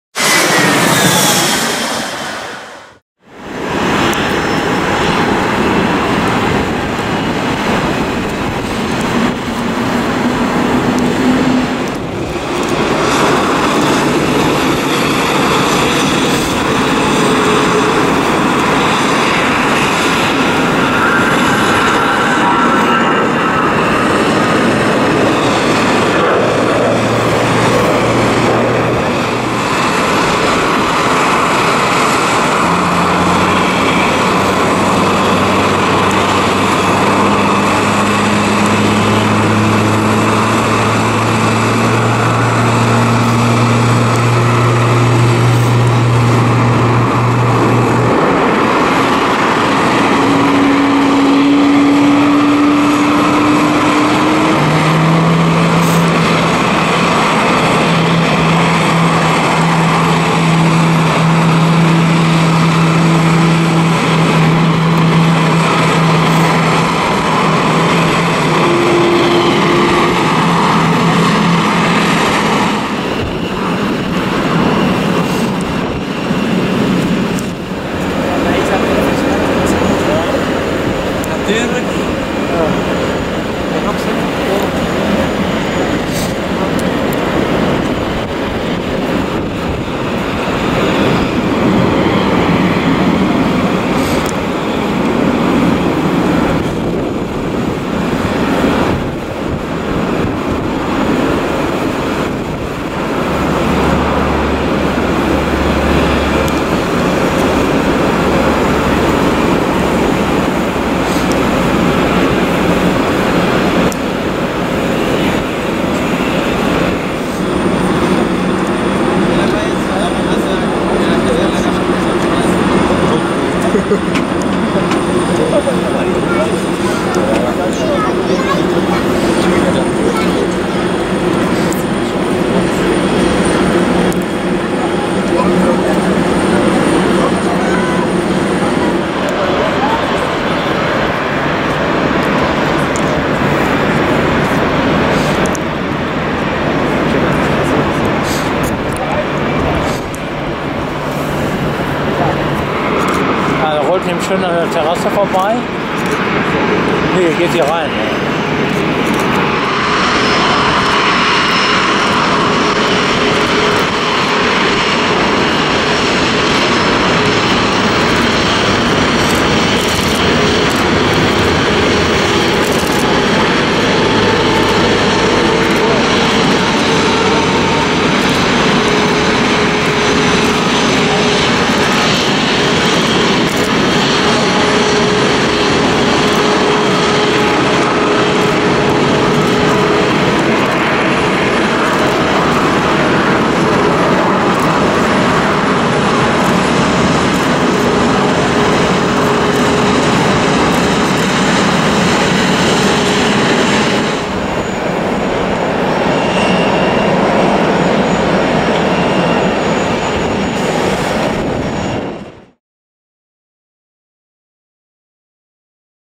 *REJECTED TAKEOFF* Cathay Pacific Airbus A350-900 at Düsseldorf Airport
Wow, I did not expect to catch this! I filmed the push-back and taxi of the amazing Cathay Pacific A350 at DUS and waited for the takeoff. The Airbus began to accelerate and suddenly turned off the runway. This means I got some great shots of the taxi back to gate! The problem is unknown, but the A350 departed about 1.5 hours later when I was already on my way back home…

Airport: Düsseldorf Airport (DUS/EDDL)
Runway: 23L
Airline: Cathay Pacific
Aircraft type: Airbus A350-941
Registration: B-LRE
Flight number: CX376
Origin-Destination: Düsseldorf Airport (DUS/EDDL) - Hong Kong (HKG/VHHH)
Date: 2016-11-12